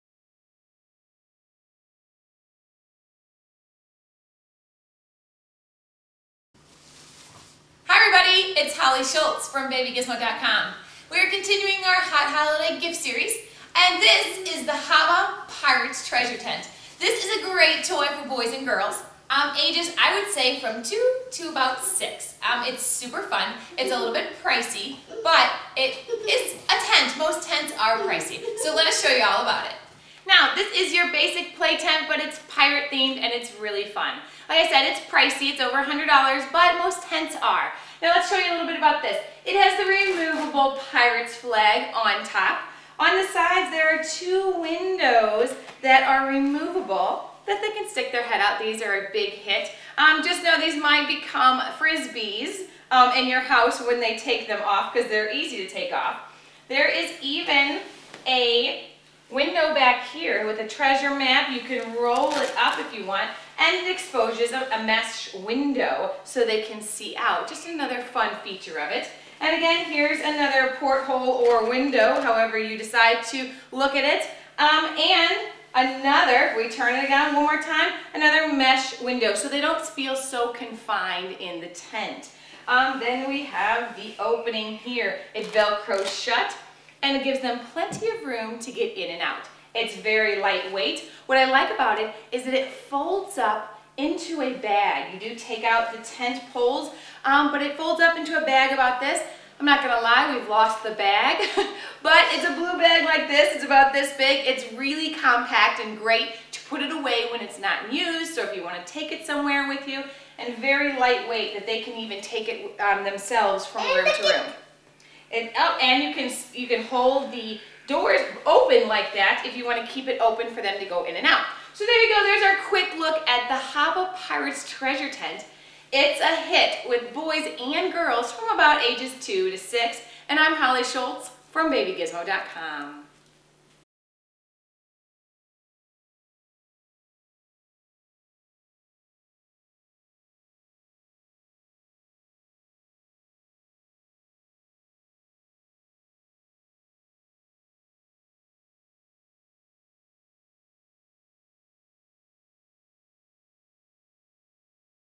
Baby Gizmo HABA Pirate's Treasure Tent Review
Hollie Schultz from Baby Gizmo highlights the HABA Pirate's Treasure Tent as part of the Baby Gizmo Hot Holiday Toys Series.